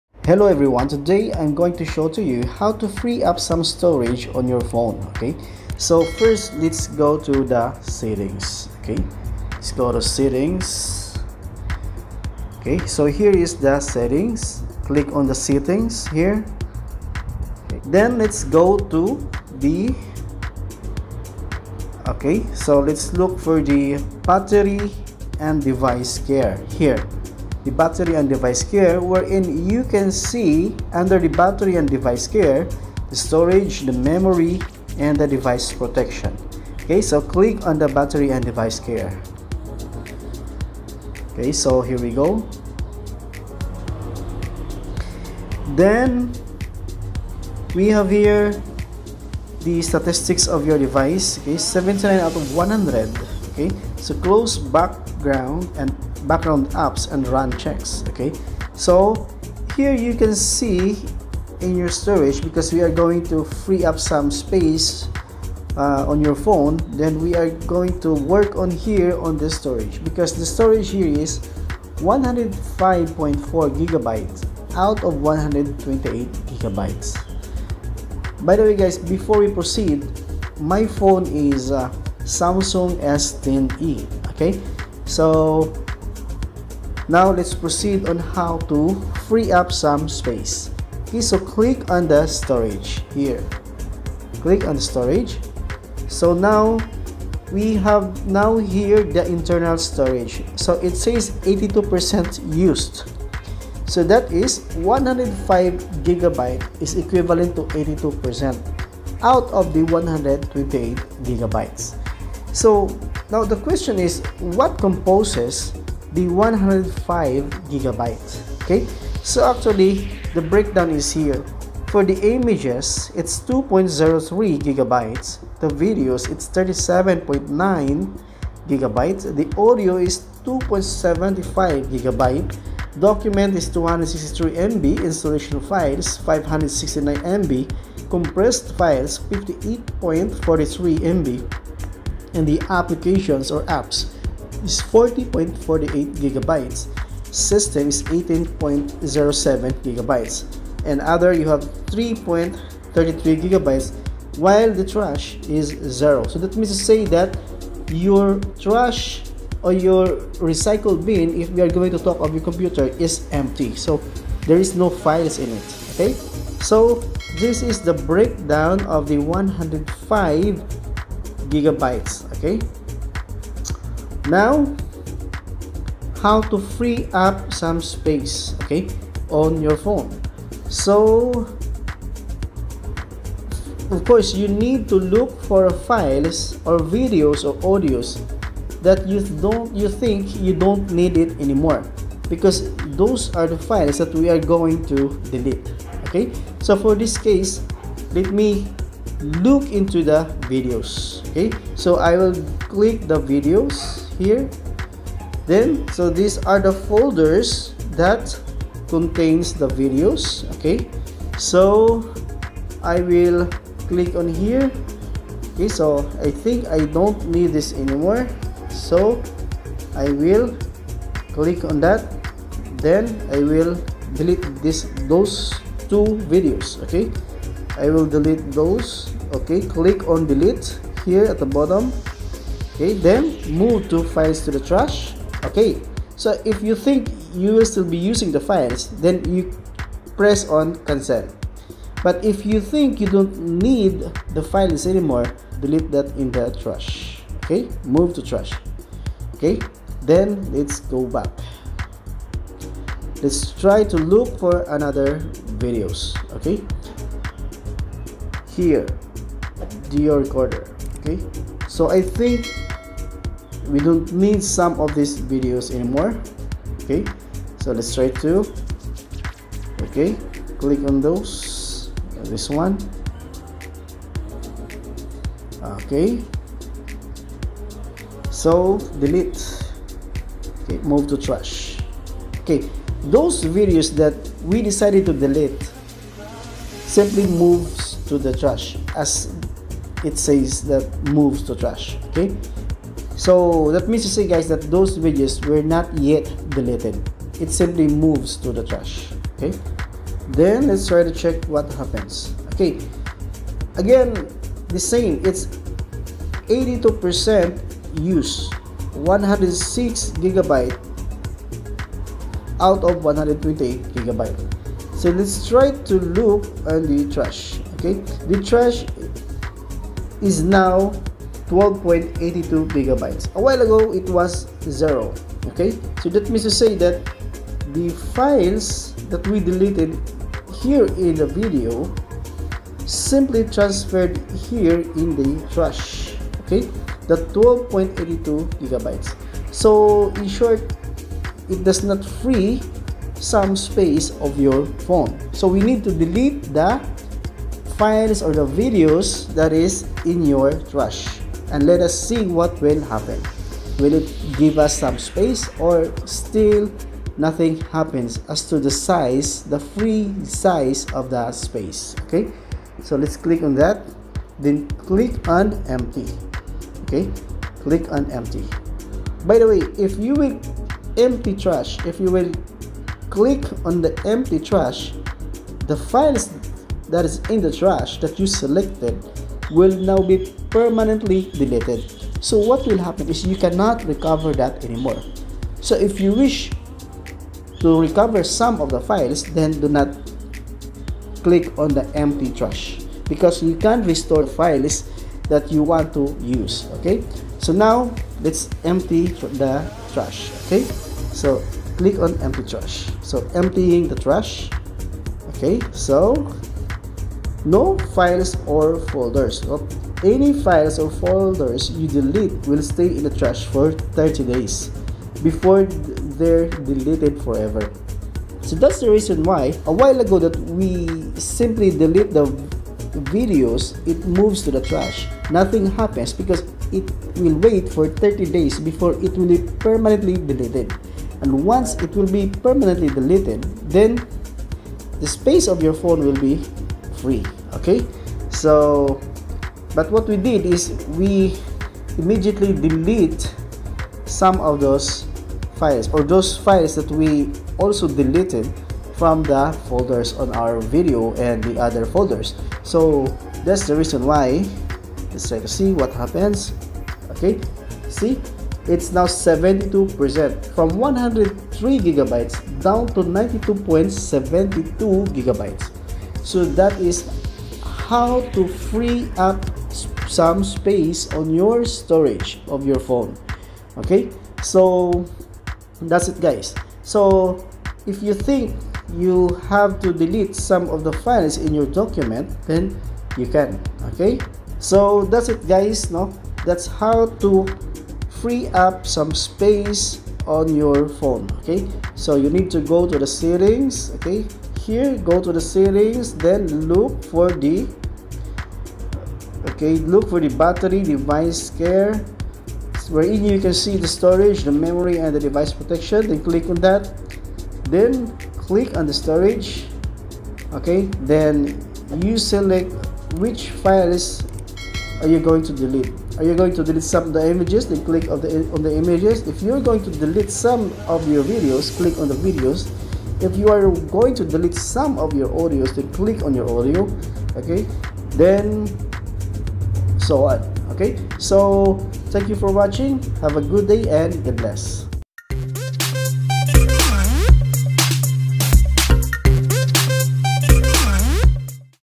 Correct Way to Free Phone Storage (Step by Step Tutorial)
Correct way delete your files to give free storage space.